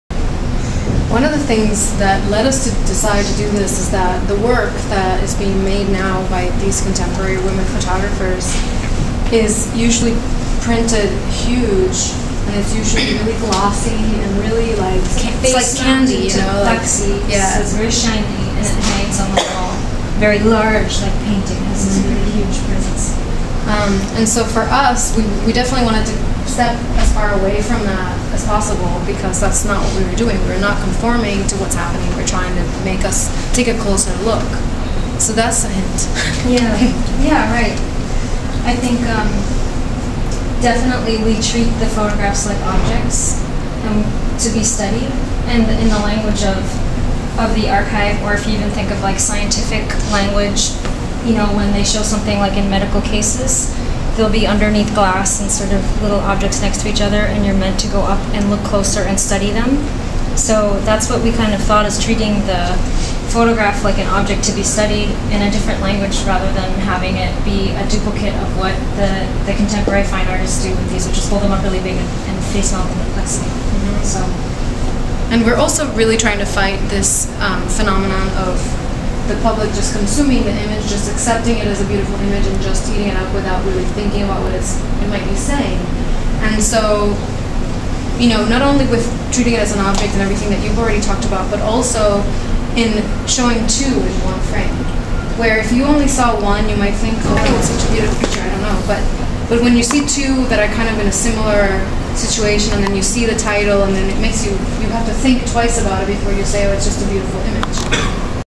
Wilka and Tarrah @ Elmira College
Artists talk with students about their collaboration (Untitled #) that is on exhibit in the George Waters Gallery, Elmira College, Elmira, NY.